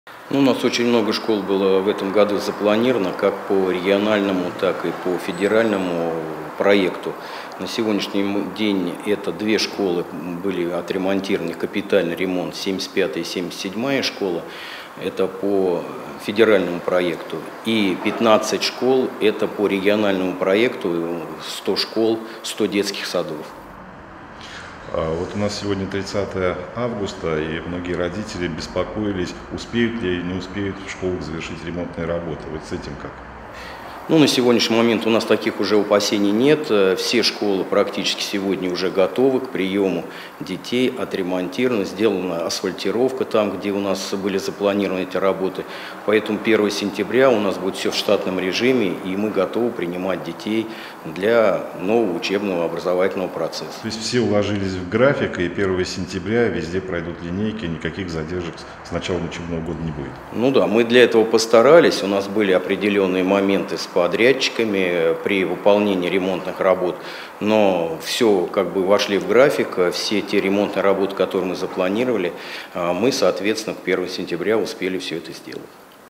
Мэрия: ремонт в саратовских школах завершен
Все саратовские школы готовы к новому учебному году. Об этом ИА «Взгляд-инфо» заявил заммэра по социальной сфере Александр Пажитнев. Опасения родителей вызывали учебные заведения, где летом шли ремонтные работы. Граждане опасались, что их не успеют завершить к 1 сентября.